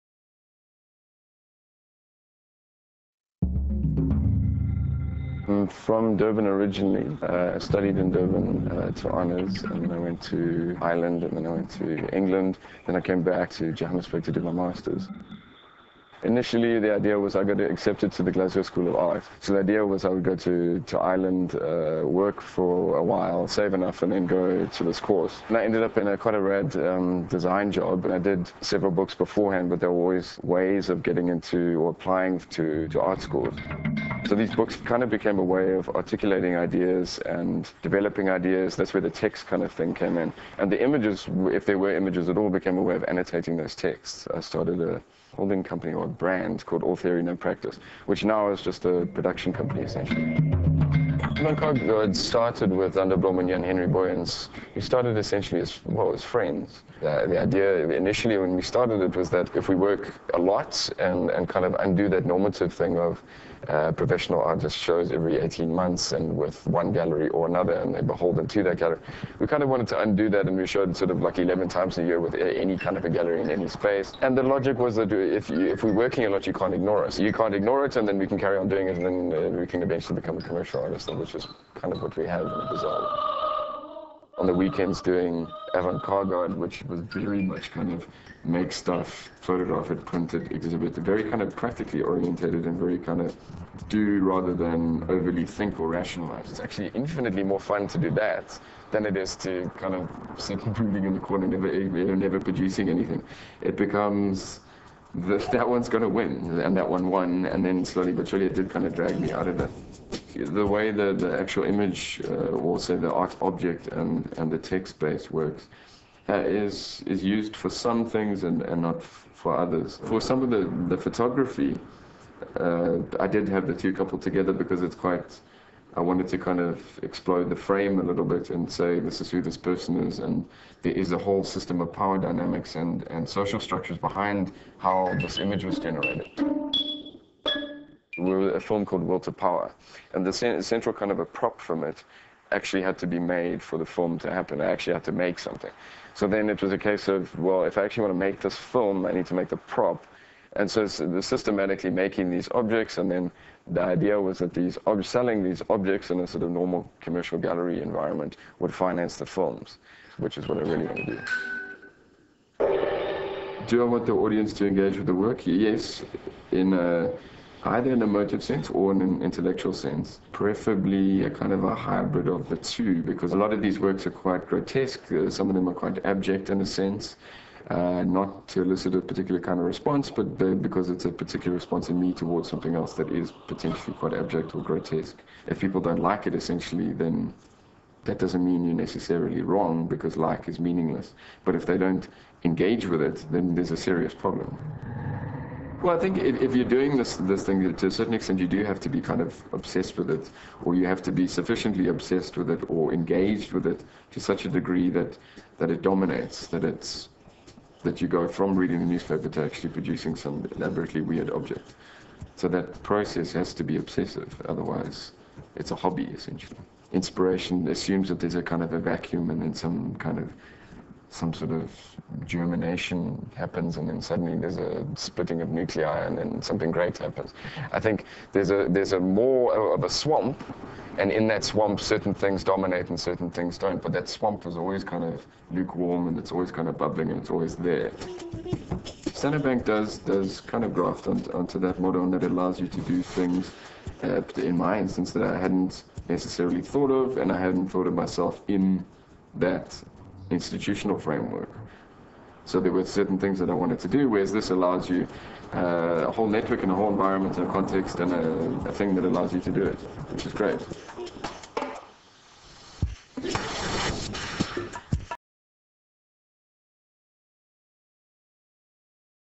Standard Bank Young Artist Award 2010 - Michael MacGarry (Visual Art)
The National Arts Festival pays tribute to the 2010 winners of the Standard Bank Young Artist Awards. Visual Art winner is Michael MacGarry. Find out how you can see Michael's work on exhibition at the 2010 National Arts Festival in Grahamstown from 20 June - 4 July 2010